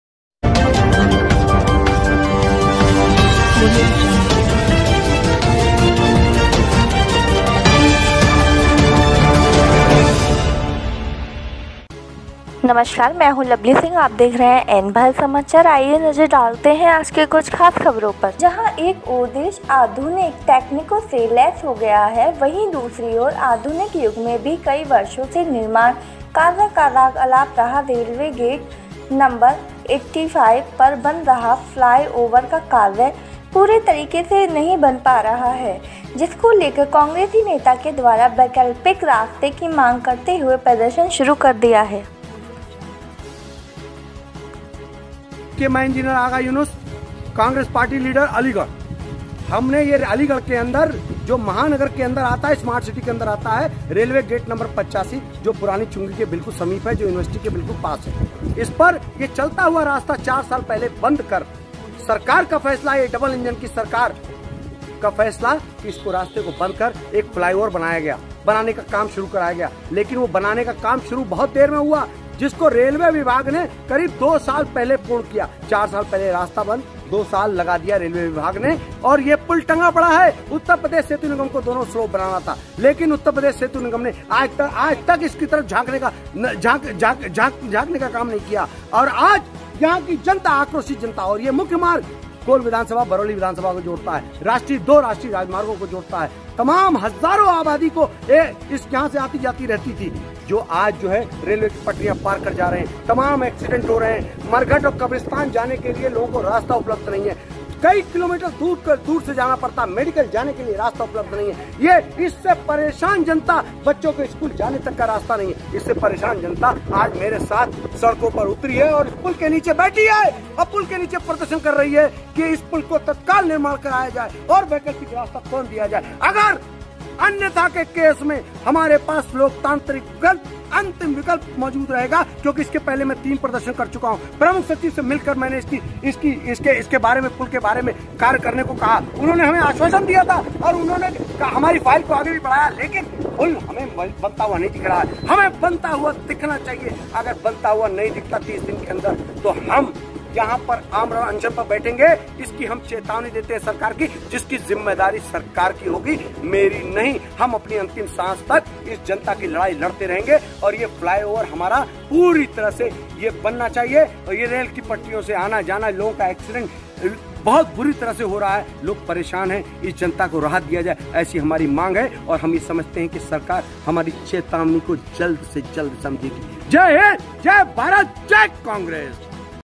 वर्षों बाद भी नहीं हुआ रेलवे गेट नंबर 85 का फ्लाइ ओवर का निर्माण,वैकल्पिक रास्ते को लेकर प्रदर्शन
स्लग---वर्षों बाद भी नहीं हुआ
रेलवे गेट नंबर 85 का फ्लाइ ओवर का निर्माण,वैकल्पिक रास्ते को लेकर प्रदर्शन सुरु

एंकर--जहां एक ओर देश आधुनिक तकनीकों से लैस हो गया है वहीं दूसरी ओर आधुनिक युग मे भी कई वर्षों से निर्माण
 कार्य का राग अलाप रहा अलीगढ़ के रेलवे गेट नंबर 85 पर बन रहे फ्लाई ओवर का कार्य पूरे तरीके से नहीं बन पारहा,
जिसको लेकर कांग्रेसी नेता के। द्वारा वैकल्पिक रास्ते की मांग करते हुए प्रदर्शन सुरु कर दिया  है,

वीओ---दरअसल पूरा मामला
अलीगढ़ के रेलवे गेट नंबर 85 व फिरदौस नगर से चुंगी की ओर आने वाले उस फ्लाइ ओवर का है जहां का रास्ता दो विधानसभा कोल और बरौली विधानसभा और राष्ट्रीय राजमार्ग NH 509 और दिल्ली जाने वाले राष्ट्रीय राजमार्ग और आसपास की हजारों की आबादी के आवागमन
को आपस मे जोड़ने का काम करता है,साथ ही एएमयू के छात्राओं के छात्रावास से लेकर विभाग का रास्ता भी इसी रास्ते से होकर गुजरता है, इस रास्ते को रेलवे  ने पटरी पर फ्लाइओवर बनाने के नाम पर गैर योजनाबद्ध तरीके से चालू रास्ते को चार साल पहले बंद कर दिया गया जिससे मरीजों, बच्चों, छात्राओं, बुजुर्गों, आमजनों का बुरा हाल हो गया। मरीजों का मेडिकल जाना, कब्रिस्तान, मरघट जाना तक दूभर हो चुका है।
रेलवे ने पटरी के ऊपर का अपना हिस्सा अति धीमी गति से करीब दो साल पहले बना दिया, स्लोप उत्तर प्रदेश सेतु निगम को बनाना था, जो अब तक शुरू नहीं हुआ। रेलवे ने चार साल से रेलवे फाटक का रास्ता खोलने से मना कर रास्ता बंद कर रखा है ,वहीं
काफी बार स्थानीय लोगों के द्वारा प्रार्थना पत्र दिए गए लेकिन कोई कार्यवाही नही हुई, एएमयू के छात्रों के द्वारा निर्माण कार्य को पूरा करवाने के लिए प्रदर्शन भी किया लेकिन प्रसाशन के द्वारा निर्माण कार्य जल्द पूरा करवाने का अस्वासन देकर उनको भी शांत करदिया लेकिन अब कांग्रेस के नेता आगाह यूनुस ने प्रदर्शन सुरु करते हुए वैकल्पिक रास्ते की मांग की है,आगाह यूनुस कांग्रेस के नेता के नेतृत्व मे आसपास के बडी संख्या मे लोगों के साथ धरना प्रदर्शन किया गया। और
वैकल्पिक रास्ते की मांग की गई ,कांग्रेस के नेता के द्वारा कहा गया अगर 30 दिन मे  काम होता नहीं देखा गया, तो 30 दिन बाद अनिश्चित काल अनशन पर बैठूंगा। प्रदर्शन के बाद महामहिम राष्ट्रपति महोदय के नाम संबोधित ज्ञापन जिलाधिकारी महोदय के माध्यम से थाना प्रभारी सिविल लाइन थाना को धरना स्थल पर सौंपा। जिसका जिम्मेदार उत्तर प्रदेश सेतु निगम और उत्तर प्रदेश की सरकार और रेलवे विभाग होगा।

बाइट--कांग्रेसी नेता आगाह यूनुस

मोहम्मद शाहनवाज अलीगढ़